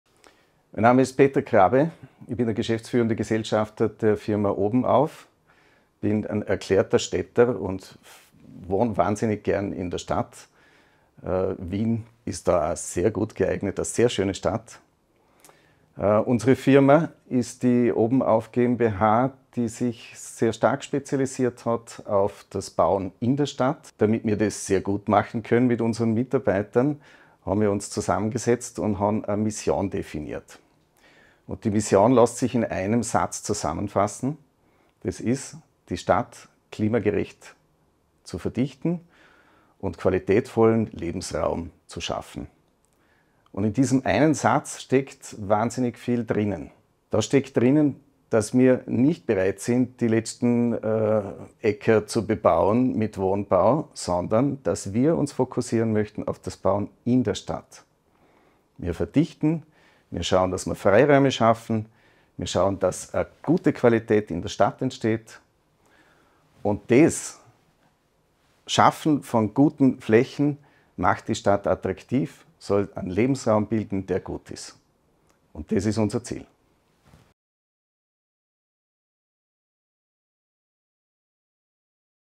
Städte klimagerecht verdichten – Dachgeschossausbau in Wien
Peter Krabbe, Geschäftsführender Gesellschafter von OBENAUF GmbH, gab im Rahmen der Veranstaltung «Stadt aus Holz» einen Einblick in die Tätigkeit des Unternehmens. Die Vision lautet: die Stadt klimagerecht zu verdichten und qualitätsvollen Lebensraum zu schaffen.